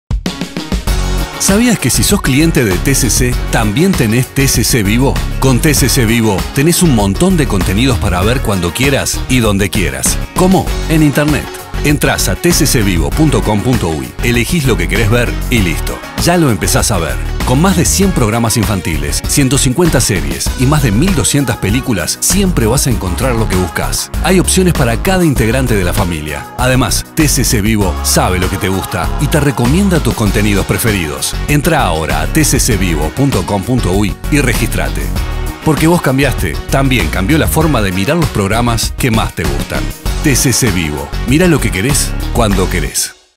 TCC Vivo. Mirá lo querés, cuándo querés.
Si tenés TCC , tenés TCCVivo.
Con TCCVivo tenés todo el contenido para ver dónde y cuándo quieras. Cine, series, programas infantiles y mucho más para que se diviertan todos.

¡Ingresá a , elegí lo que querés mirar y listo!